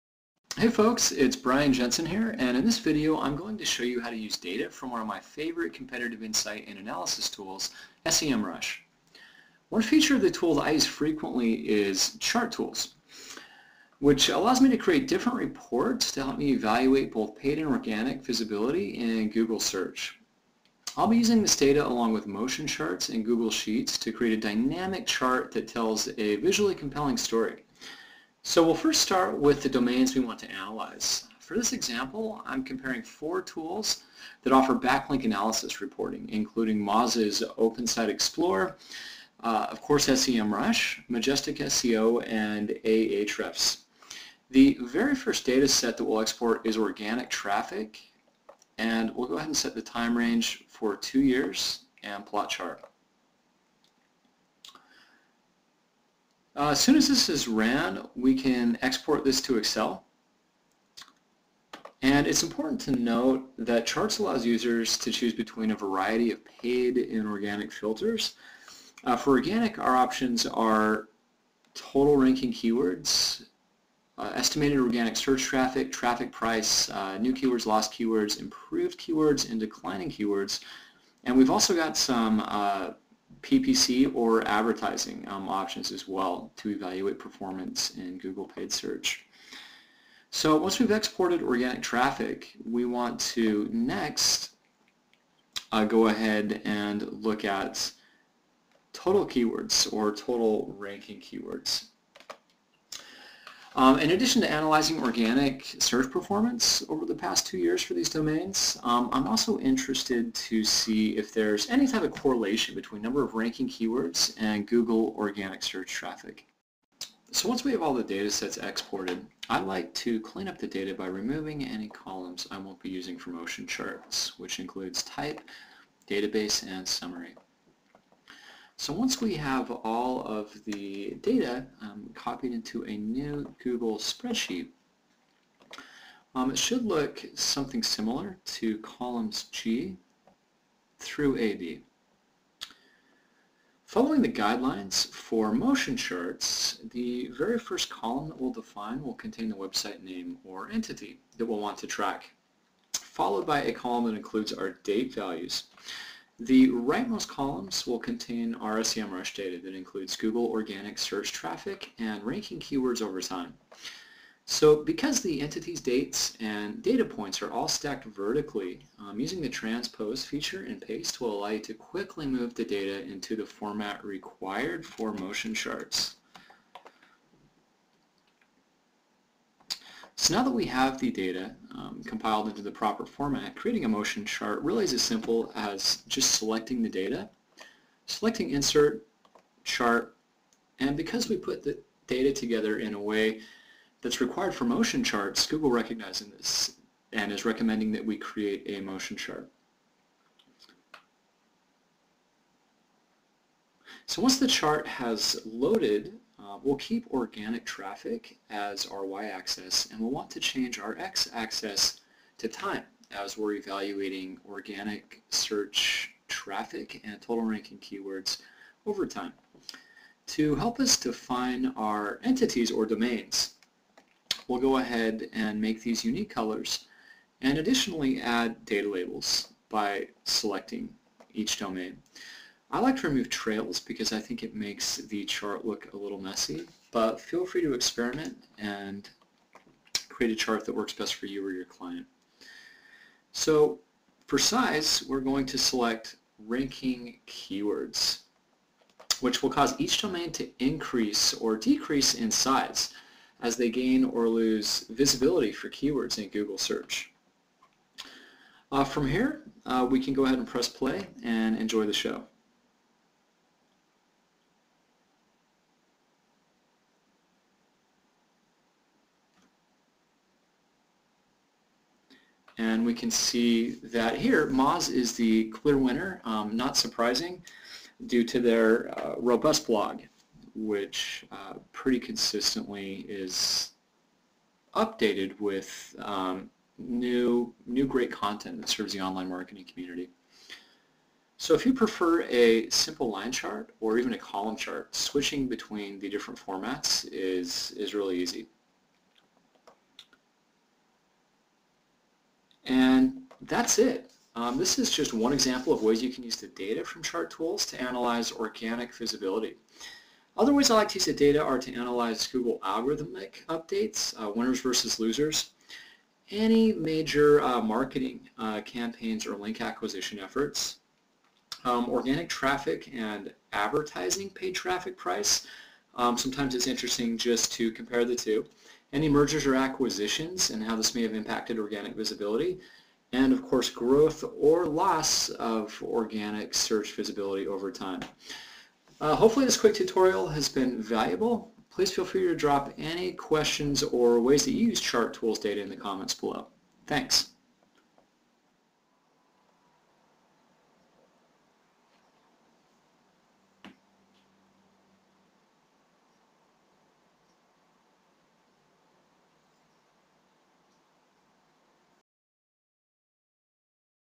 SEMrush: Using Chart Tools & Google Docs to Create Dynamic Charts
Hi folks, it's Brian Jensen here and I'm going to show you how to use data from one of my favorite competitive insight and analysis tools SEMrush.

One feature of the tool that I use frequently is chart tools, which allows me to create different reports that help me evaluate both paid and organic visibility in Google search.

I'll be using this data along with motion charts in Google Sheets to create a dynamic chart that tells a visually compelling story.

We'll first start with the domains we'll want to analyze. For this example, I'm comparing four tools that offer backlink analysis reporting including Open Site Explorer, Ahrefs, Majestic SEO and SEMrush. The first data set we'll export is Organic Traffic and we'll set the time range to two years.

The next data set we'll export is number of total ranking keywords because in addition to analyzing organic search performance over the past two years for these domains, I'm interested to see if a correlation exists between number of ranking keywords and Google organic search traffic.

It's important to note that Charts allows users to choose between a variety of paid and organic filters. For organic, our options are
organic search traffic, total ranking keywords, traffic price, new keywords, lost keywords, improved keywords and declining keywords.

Once we have both data sets exported, I like to clean up the data by removing any columns I won't be using including type, Database, and summary. Once we have all SEMrush data into a new Google Sheet, it should looks similar to Colums G through AD.

Following the guidelines for motion charts, the first column we’ll define  will be contain the website name (entity) that we’ll
want to track, followed by a column that includes date values (this chart supports multiple date formats).  The rightmost columns will contain our SEMrush data that includes Google organic search traffic and ranking keywords overtime. Because entities, dates, and data points are all stacked vertically, using the transpose feature in paste will allow you to quickly move the data into the format required for Google motion charts.

Once the chart has loaded, we’ll select Organic Traffic as our Y axis and Time as our X axis.  To help us define our entities, we’ll select all domains and additionally select unique colors from the color menu.  For size, select Ranking Keywords which will cause each domain to increase or decrease in size as they gain or lose visibility for keywords in Google search. From here, I'll select the competitor, remove trails because I think it makes the chart look messy, press play and enjoy the show.

If you prefer a simple line or column chart, switching chart formats in Google Sheets is as simple as choosing between your chart preferences for visually displaying the data.

Another awesome feature worth mentioning in Google Sheets is the ability to publish your chart, which creates a URL for easy sharing between co-workers and clients.

This is just one example of ways you can use data from Charts to analyze organic visibility and trends. Other ways i use the data are to analyze

• Google algorithm updates – winners vs. losers
• Any major marketing campaigns, or link acquisition efforts – most backlink analysis tools allow you to analyze links acquired over time.
• Organic traffic and advertising paid traffic price – how much traffic a competitor is acquiring organically and how much they are paying for clicks
• Mergers and acquisitions and how this many have impacted organic visibility
• Growth or loss of visibility over time

Hopefully this quick tutorial has been valuable. Please feel free to drop any questions or ways you use chart tools data in the comments below.